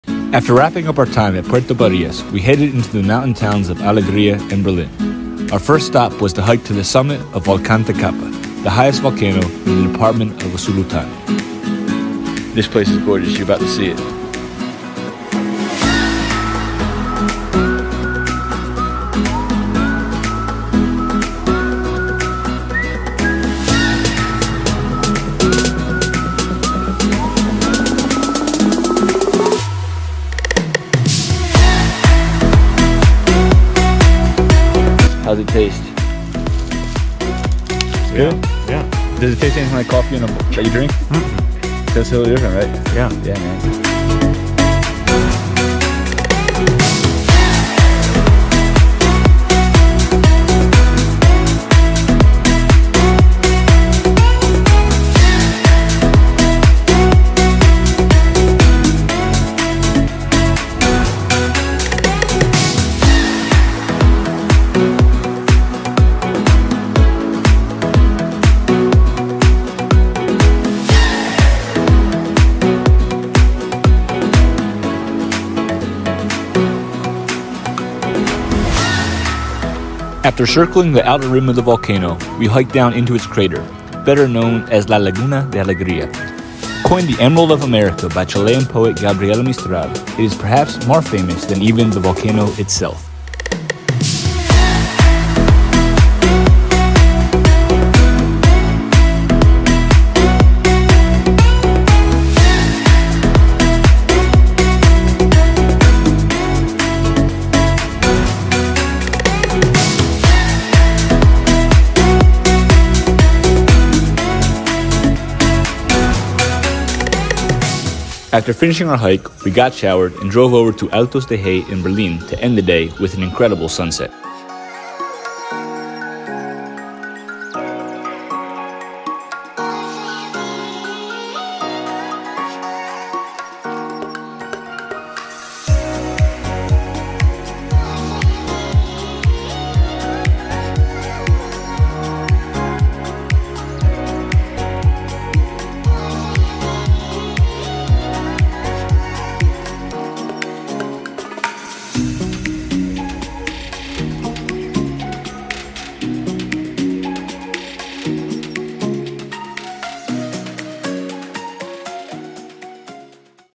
Hiking the Highest Volcano in Usulutan, El Salvador
Continue traveling with us through Usulutan, El Salvador as we visit Volcan Tecapa, La Laguna de Alegria, and Altos de Jey in the towns of Alegria and Berlin.

Timestamps:

Volcan Tecapa 0:00
Laguna de Alegria 1:20
Altos de Jey 1:58